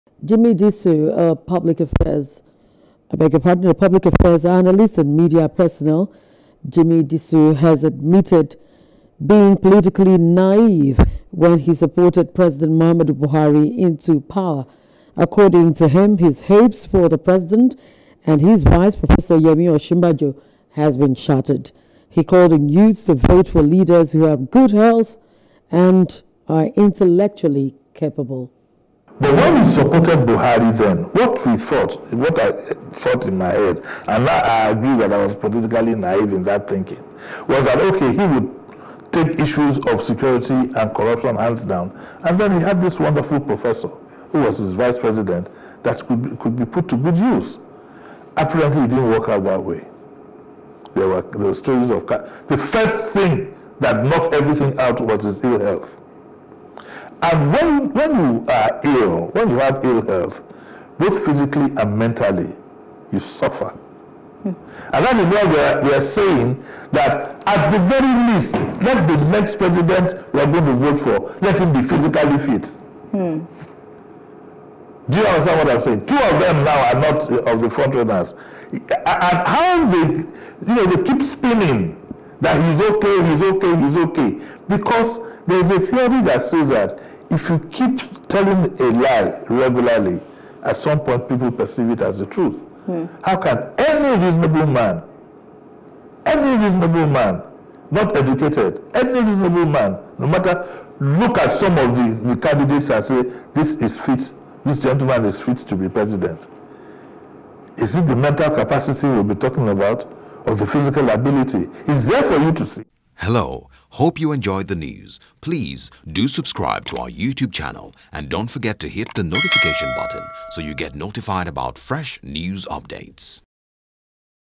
"I Was Politically Naive When I Supported Buhari" – Jimi Disu
Jimi Disu , a public affairs officer had admitted being politically naïve when he supported president Buhari into power.

 According to him, his hopes on the president, supported by his vice , yemi osinbajo, who is a professor was shattered.